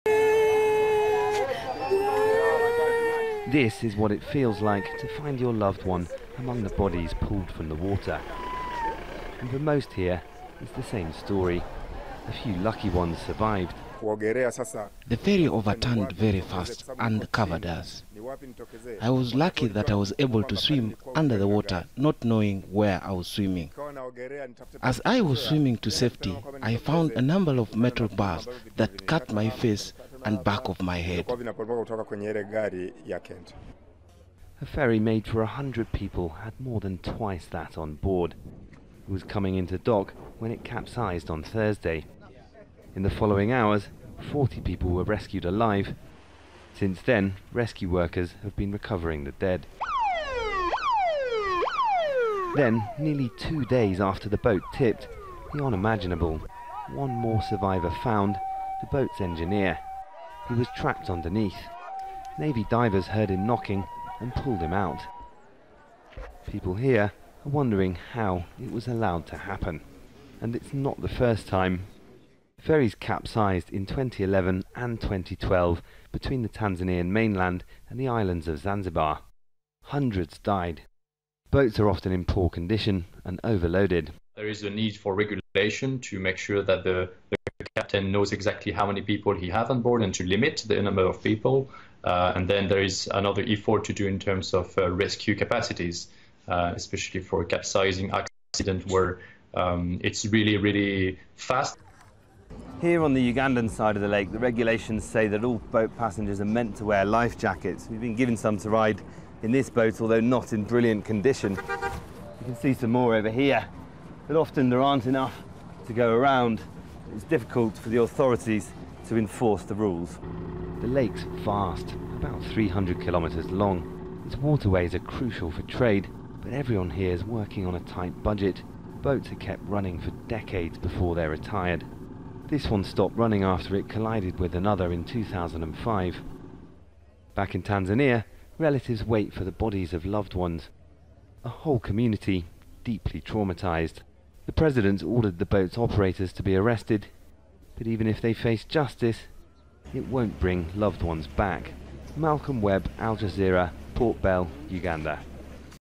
🇹🇿 Tanzania ferry disaster: Survivor found as death toll passes 200 l Al Jazeera English
A survivor was found in an air pocket inside the capsized Tanzanian ferry on the third day of a grim recovery operation.

Four navy divers heard sounds that suggested signs of life early on Saturday and were able to pull the man out of the ship and rush him to hospital. His condition was not immediately known.

The death toll from the disaster has passed 200, the rescue brings the number of survivors to 41.

Al Jazeera’s Malcolm Webb reports from Port Bell on the Ugandan side of the lake.